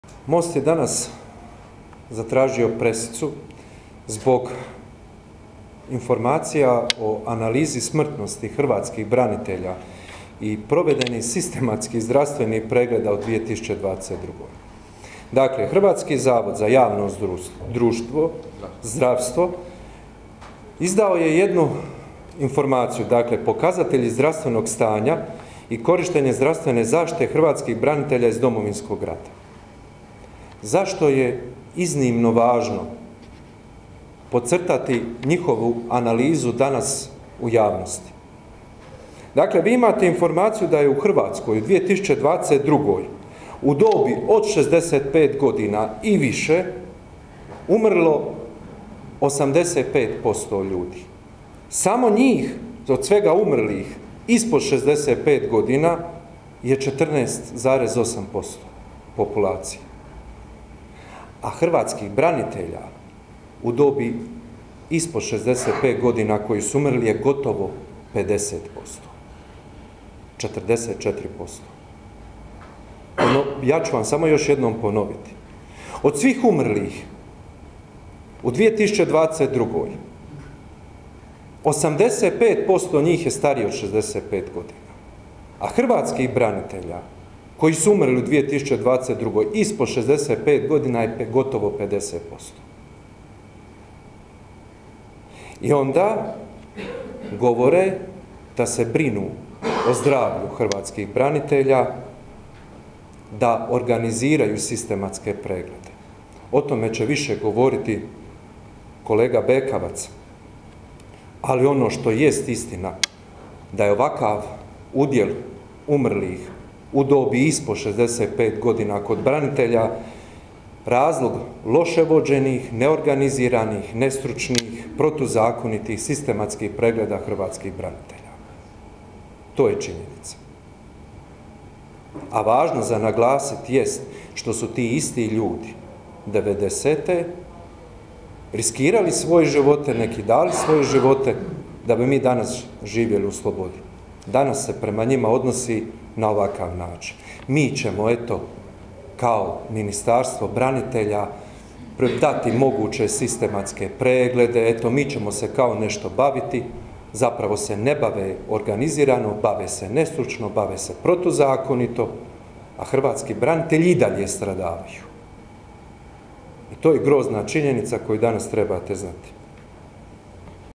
Petrov: "Hrvatski branitelji i dalje stradavaju"
Na konferenciji za medije Mosta održanoj u Saboru o temi  "Branitelji Domovinskog rata žrtve su površnosti nadležnih ministarstava u pripremi i provedbi sistematskih pregleda” govorili su Božo Petrov i Ivan Bekavac